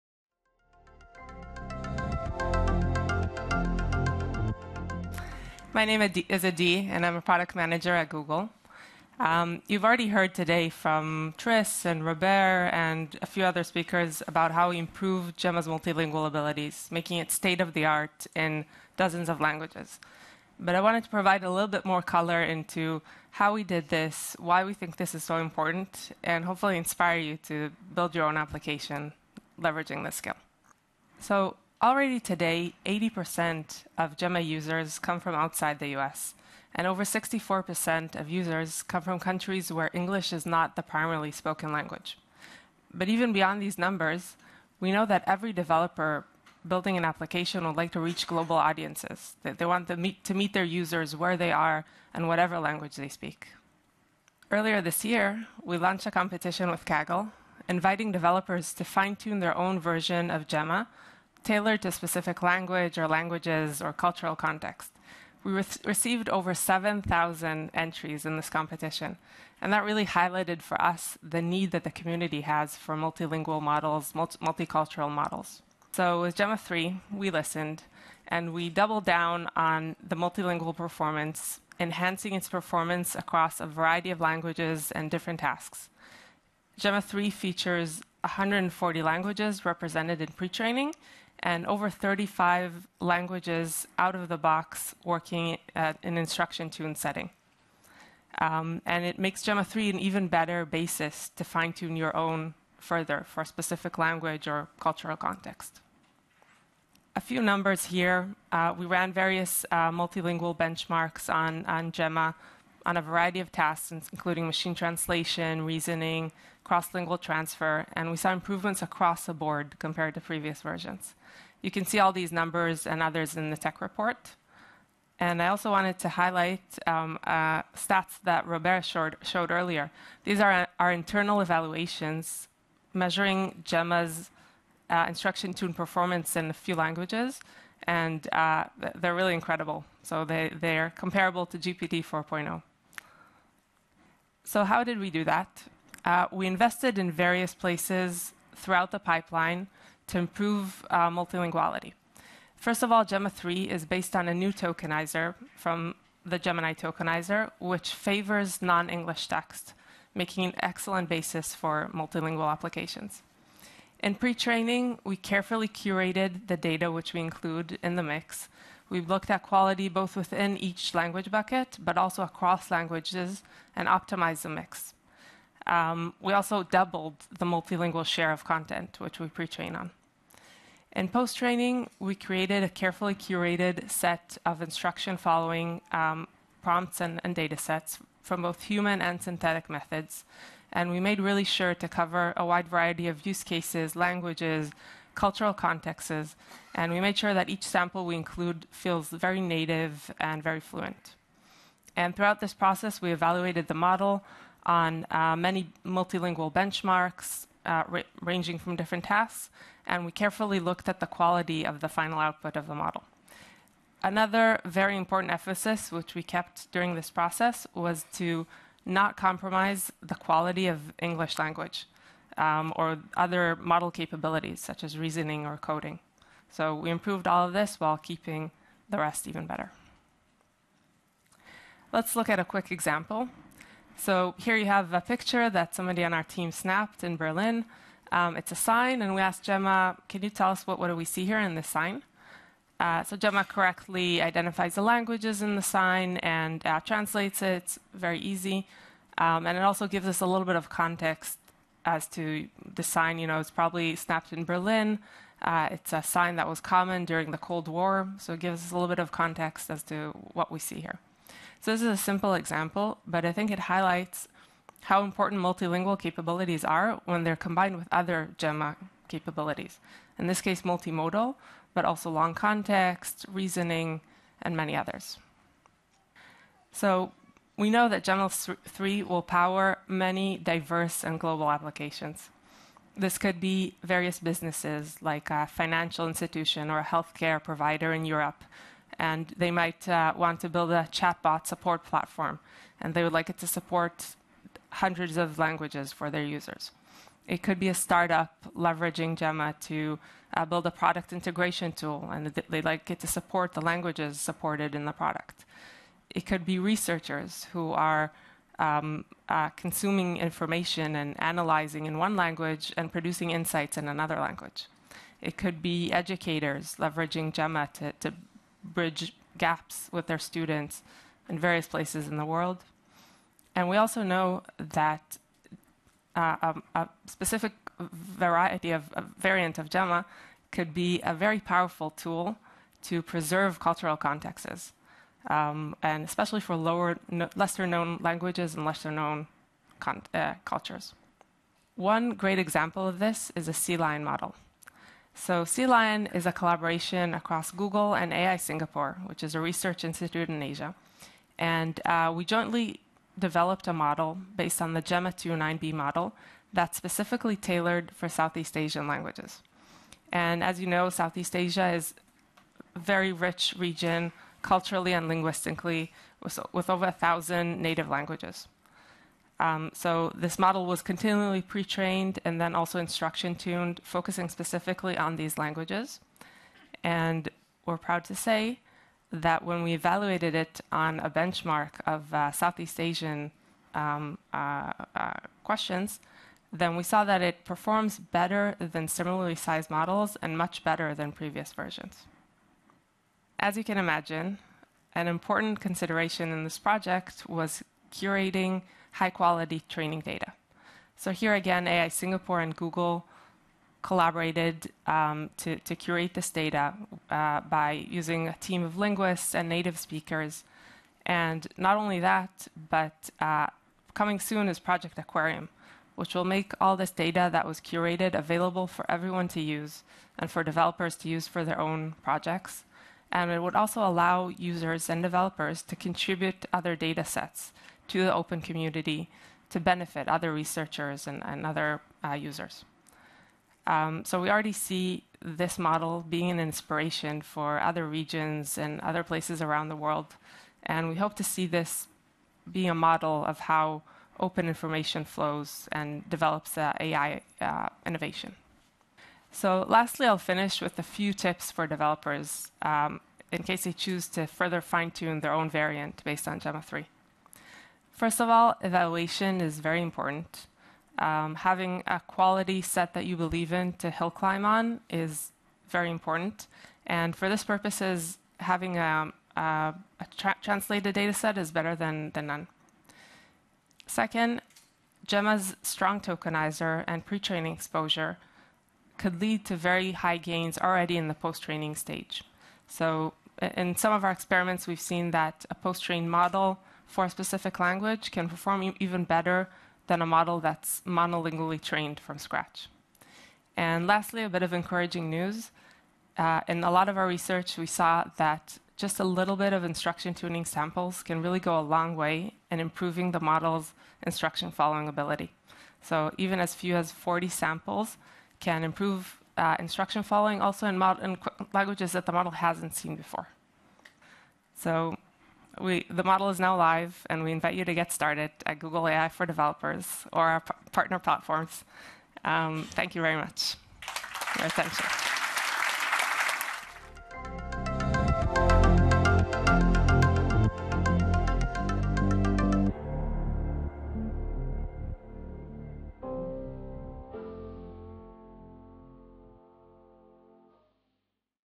A truly multilingual Gemma 3
Building multilingual AI applications is crucial for reaching global audiences, and varied language proficiency remains a top developer priority. In this talk, we’ll explore the training strategies applied to make Gemma 3 fluent in over 100 languages, and showcase its state-of-the-art performance on multilingual tasks.

Speakers: Adi Mayrav Gilady
Products Mentioned: Gemma